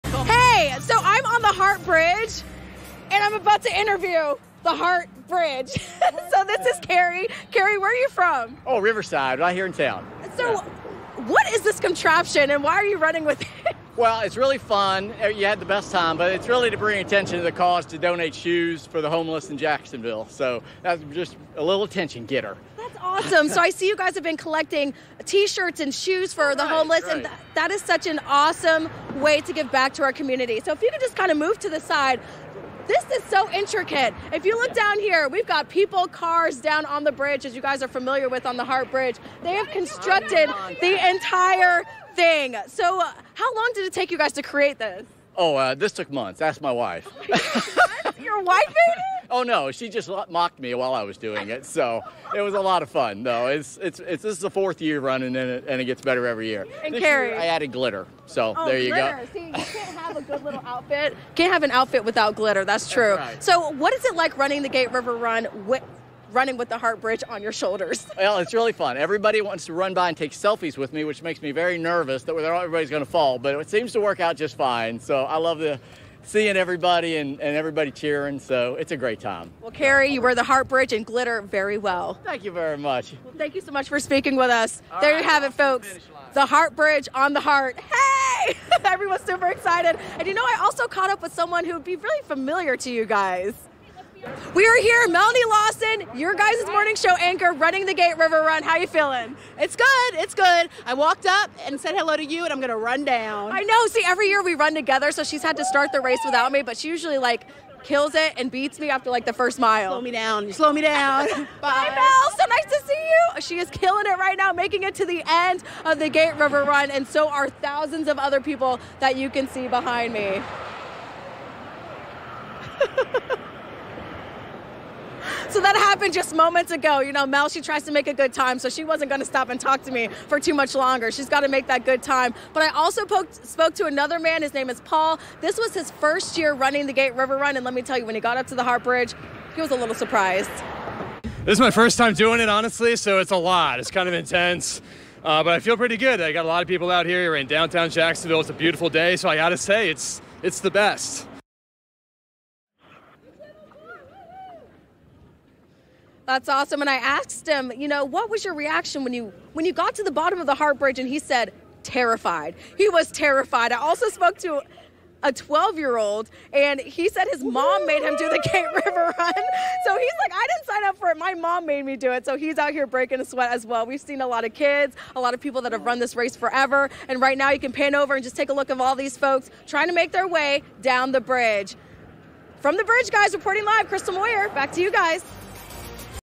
Gate River Run from the Hart Bridge
Crystal Moyer joins us from the Hart Bridge where she talks with some runners during the Gate River Run, and finds a familiar face!